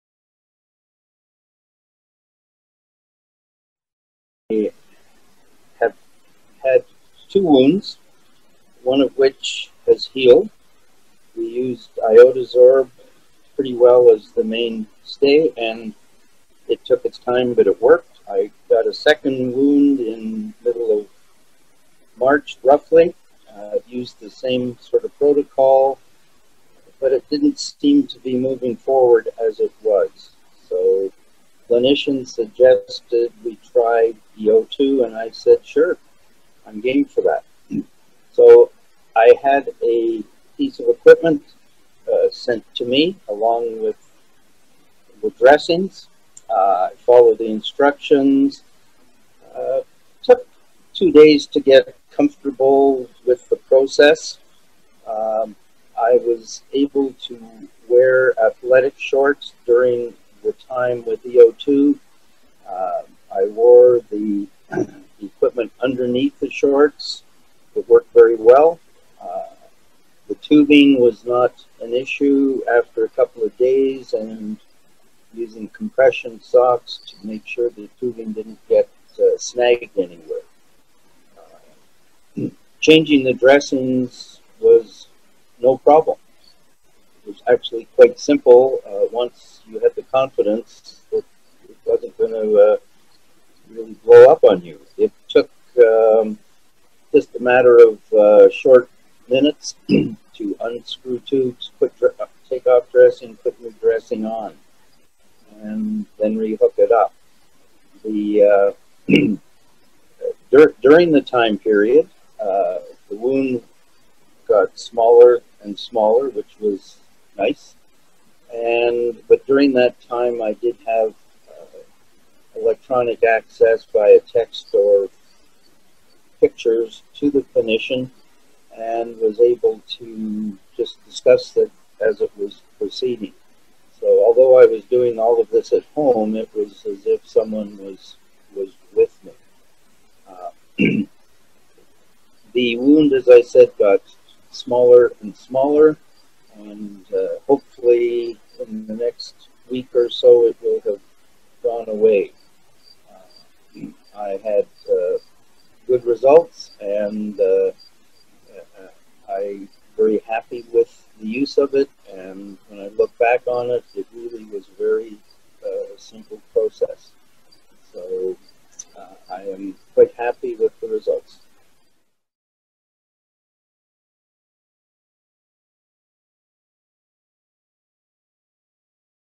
CDO Therapy Patient Testimonial - Bill Murray
Patient of CDO Therapy, Bill describes his experience of changing his own dressings at home independently, and details of how he integrated this therapy system into his everyday life. In this video Bill was still on therapy; however, he did go on to heal in 3 months.